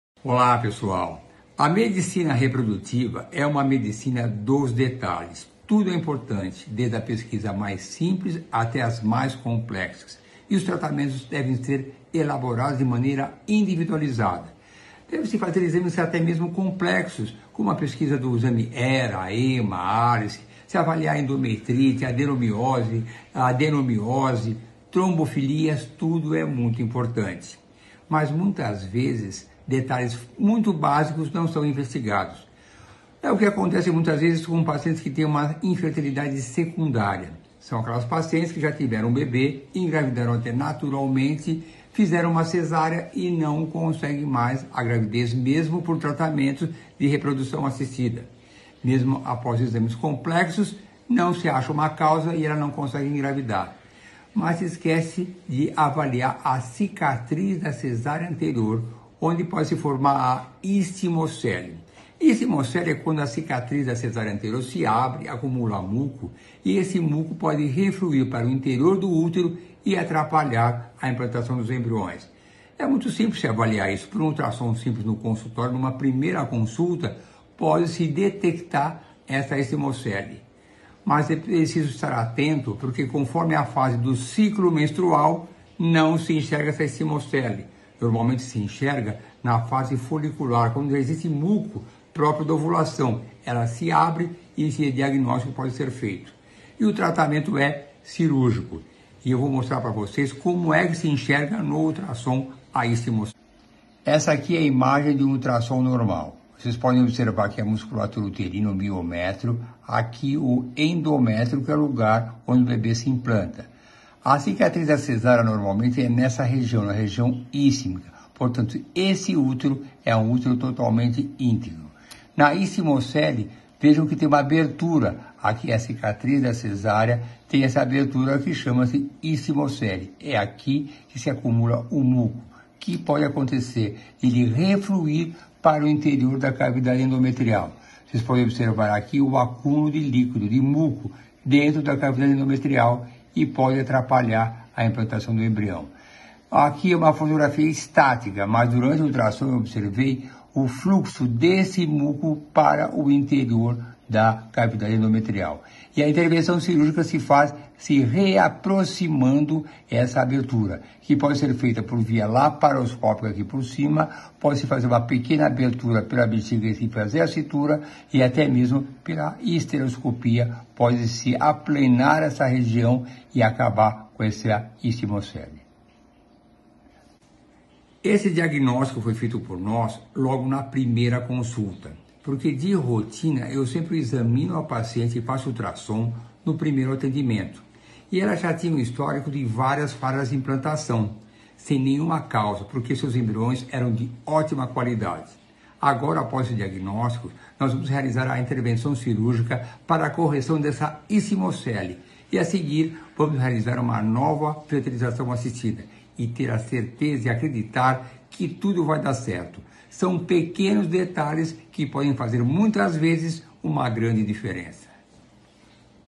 O DIAGNÓSTICO NA PRIMEIRA CONSULTA - EXAMINAR A PACIENTE É FUNDAMENTAL
A ISTMOCELE !! Não há dúvida que aplicar os avanços tecnológicos na medicina reprodutiva é muito importante. Exames como ERA, EMMA, ALICE, pesquisa genética, adenomiose, endometrite, células NK BRIGHT e DIM , trombofilias e fatores imunológicos entre outros devem fazer parte não devem ser colocados de lado, e SIM, incorporados na pesquisa avançada! O que é IGUALMENTE IMPORTANTE e jamais ser deixado de lado, é o exame ginecológico da paciente logo na primeira consulta, pois nesta primeira avaliação já é possível se obter diagnósticos incríveis com bastante precisão! Esta paciente que já tinha feito vários tratamentos sem sucesso, no primeiro atendimento, foi detectado esta alteração da anatomia uterina que poderia ser a razão do insucesso dos seus tratamentos! Uma intervenção cirúrgica MINIMAMENTE INVASIVA é capaz de resolver este problema e aumentar as chances de sucesso no próximo tratamento. EXAMINAR A PACIENTE é FUNDAMENTAL!